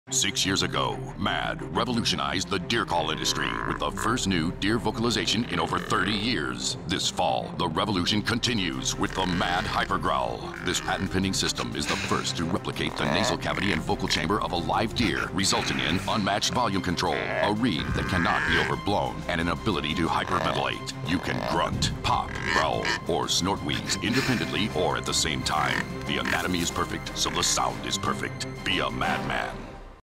MAD Hyper Growl Commercial
The revolutionary new deer call that exactly replicated the sounds real deer make!